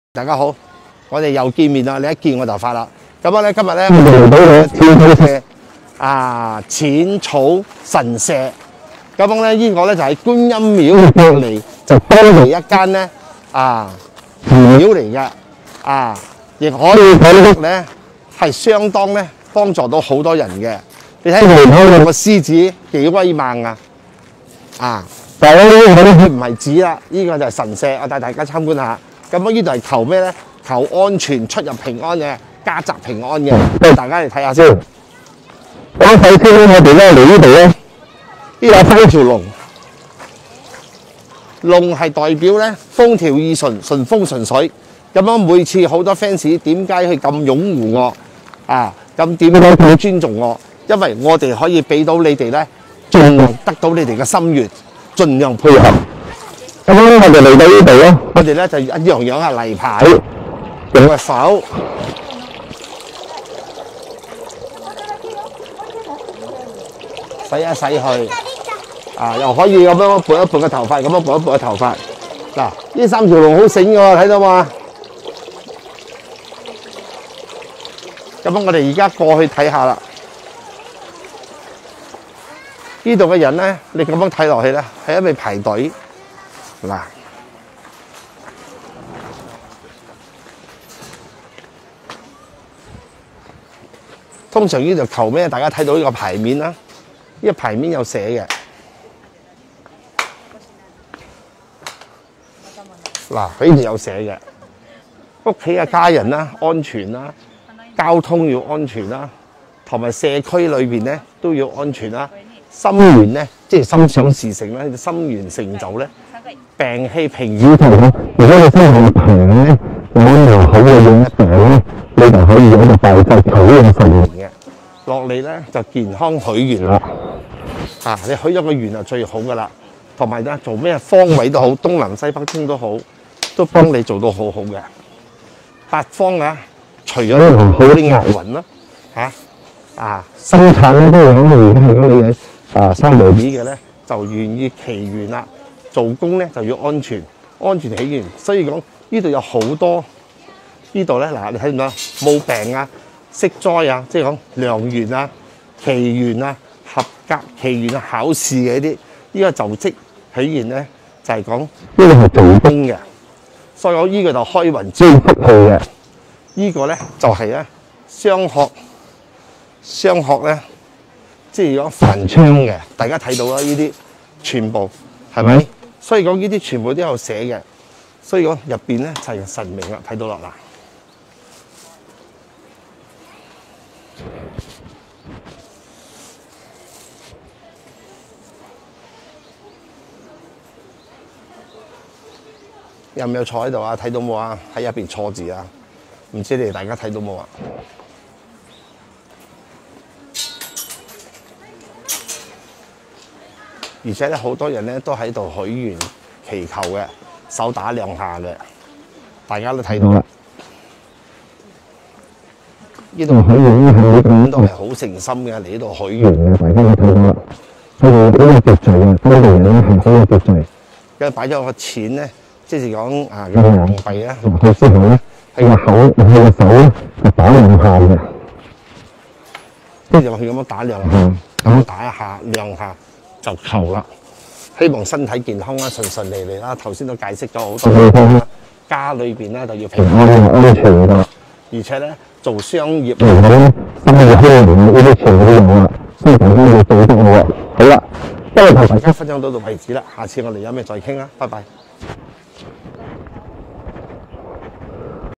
金龍王高天霸師父於日本東京淺草神社景點分享(現場錄影，如有雷同，純屬巧合，只供參考)
金龍王高天霸師父於日本東京淺草神社景點分享
(現場錄影，如有雷同，純屬巧合，只供參考)
Commentary & sharing of Golden Dragon King Master at Tokyo Asakusa Shrine, Japan.
(Live recording, any resemblance is purely coincidental, for reference only)